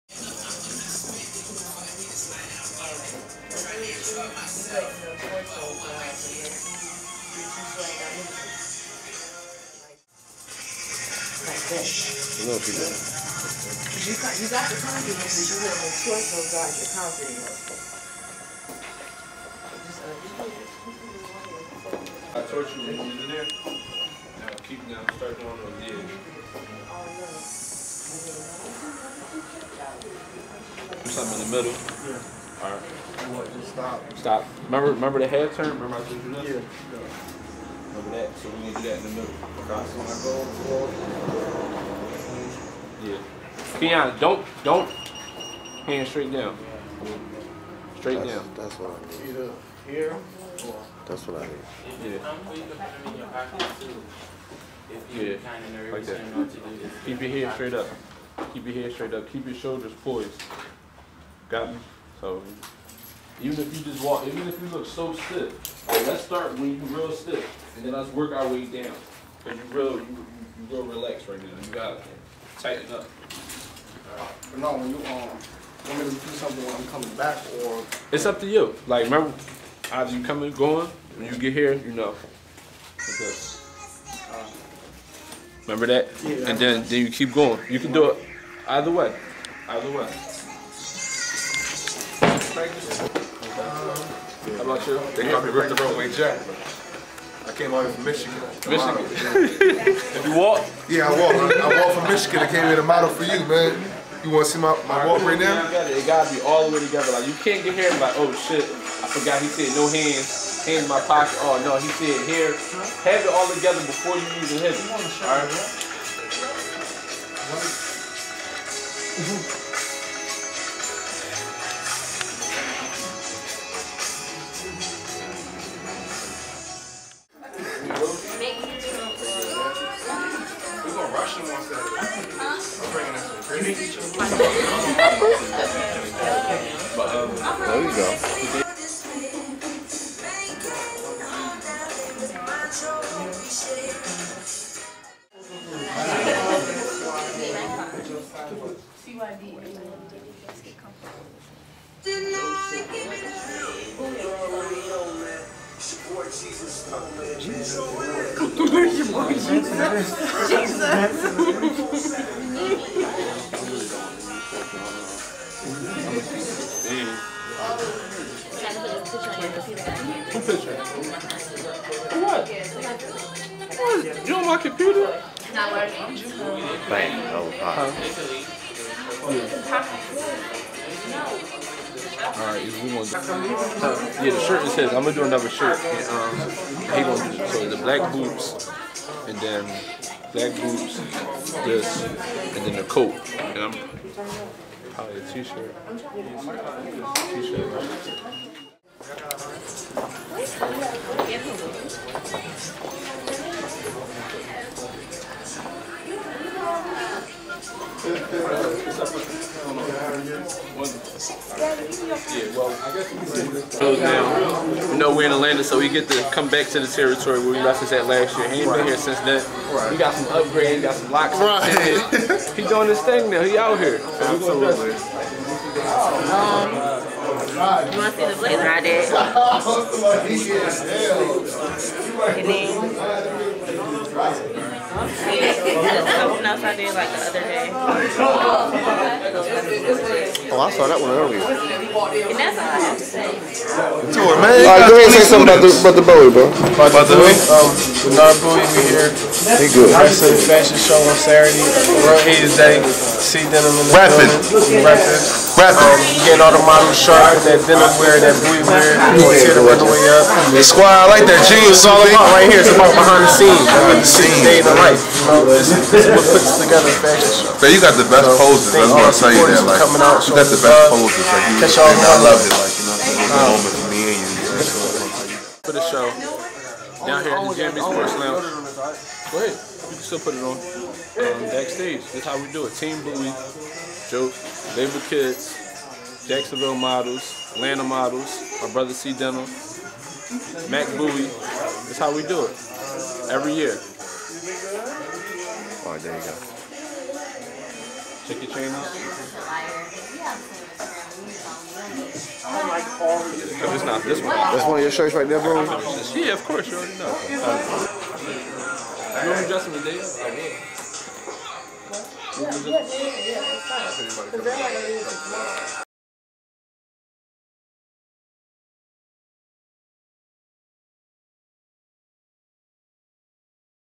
Worlds Aids Day Practice
One day before Worlds Aids Day Team Buie partnered up with PS Management from Jacksonville FL to bring the world our 1st annual Worlds Aids Day Fashion Show. You are watching the behind the scene footage of the night before the show. Special thanks to GME Sports Lounge, all our product sponsors, hair and make-up artist, video and photo teams. A Star was Born Life Save pt 6 coming soon ...